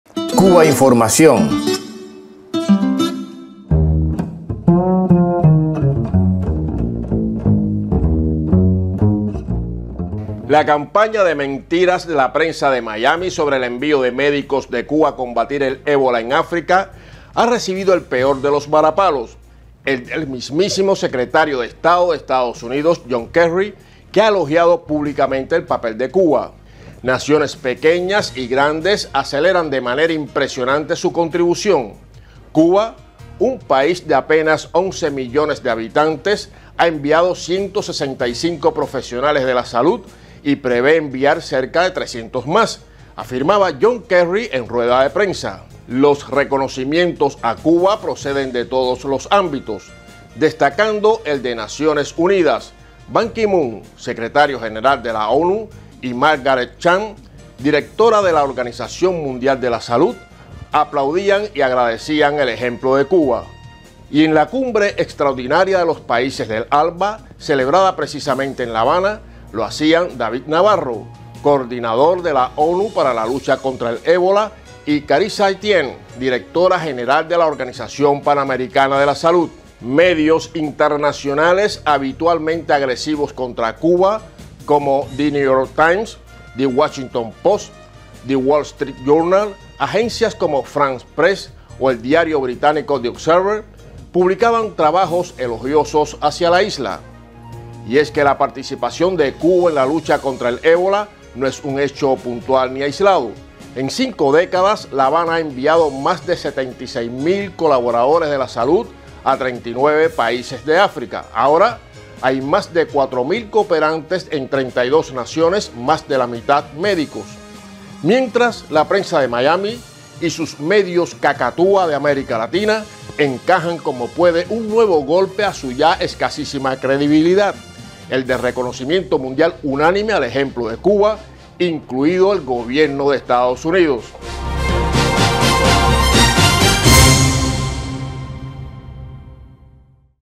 Golpe a las mentiras de Miami: hasta John Kerry elogió el papel de Cuba frente al ébola en África
Cubainformación TV.- Basado en un texto de Cubadebate.- La campaña de mentiras de la prensa de Miami sobre el envío de médicos de Cuba a combatir el ébola en África ha recibido el peor de los varapalos: el del mismísimo Secretario de Estado de EEUU John Kerry, que ha elogiado públicamente el papel de Cuba. Edición: José Ignacio Antón. Fuente